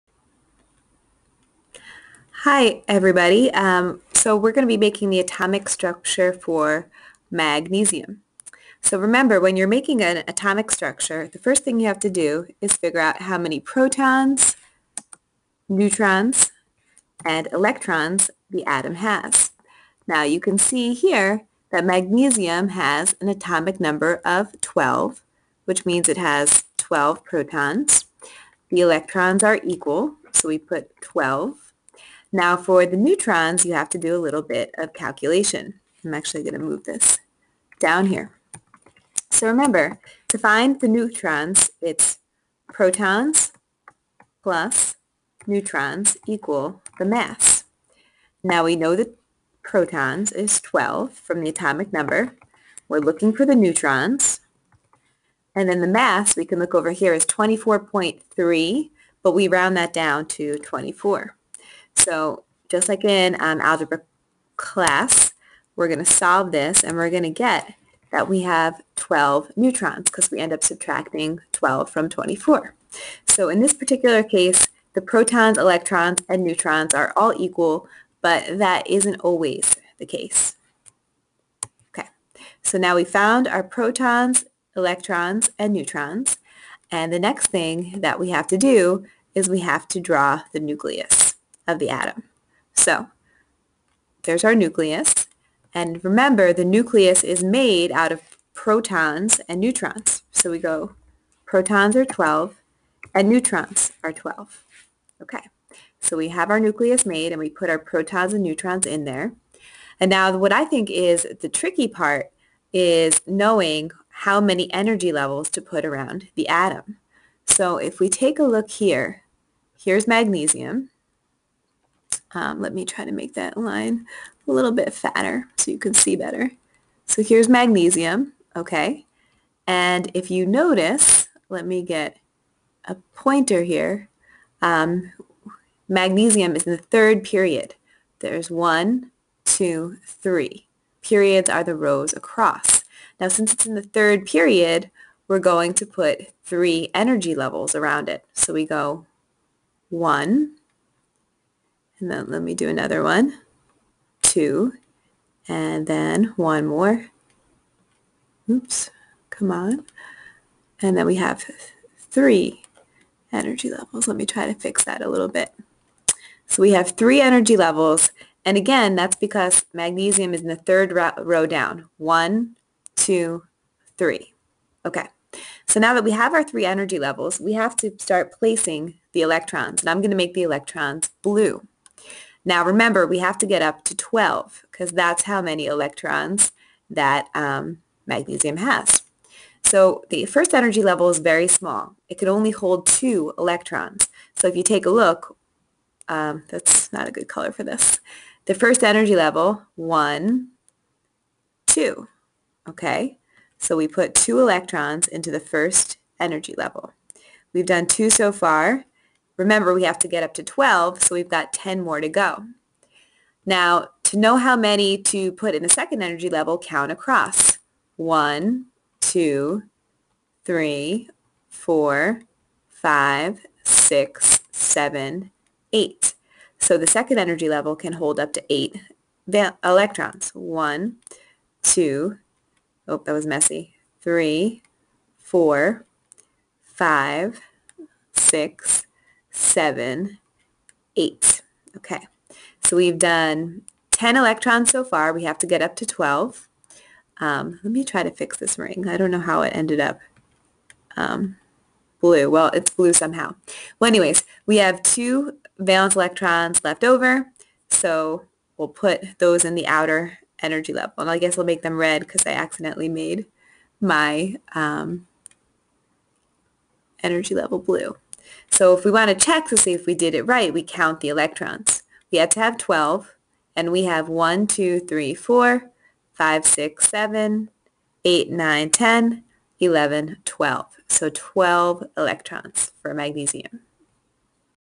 Day 8 The Atomic Structure of Magnesium
This video will show you how to draw the atomic structure of magnesium.